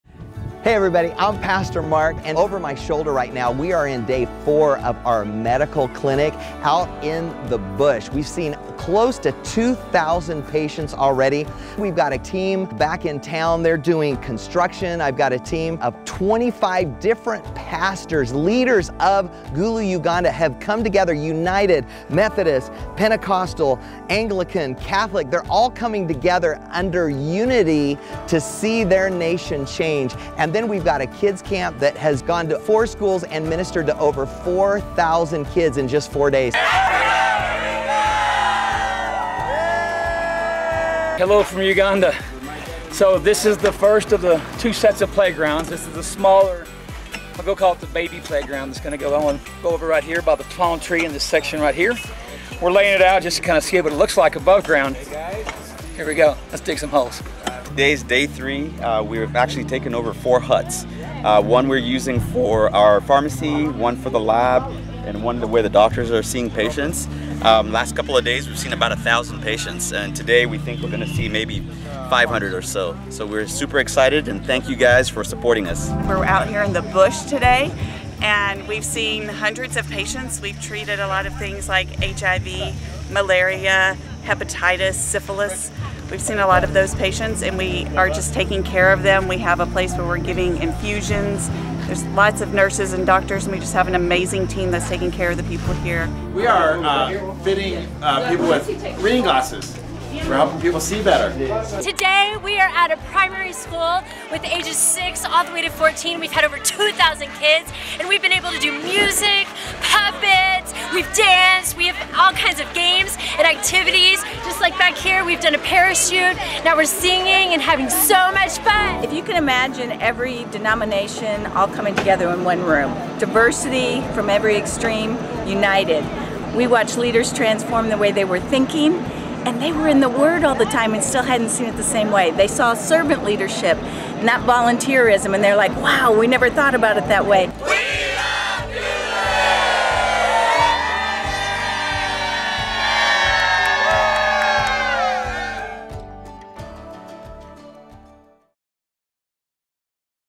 World Missions Adventure Promo Video
This Video was during one of our Missions Trips to Uganda Africa.  We did a Medical Clinic, Kids Camp, Leadership Training and built a Playset for the Children of Gulu Uganda.  This is what a Trip with World Missions Adventure is all about.